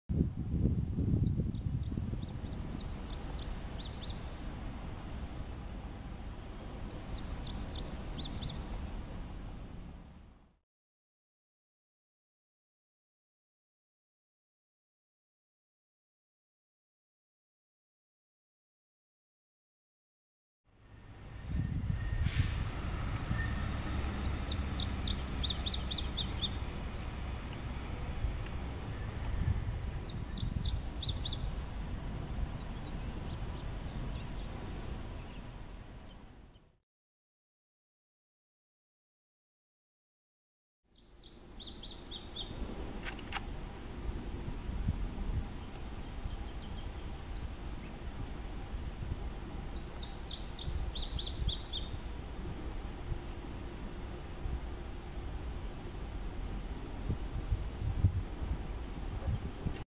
Indigo Bunting - August 6, 2019
On Tuesday. August 6, 2019. in Freetown, Massachusetts, during a Bioreserve Birding field trip with Lynn Abbey, I filmed Indigo Buntings (Passerina cyanea) singing.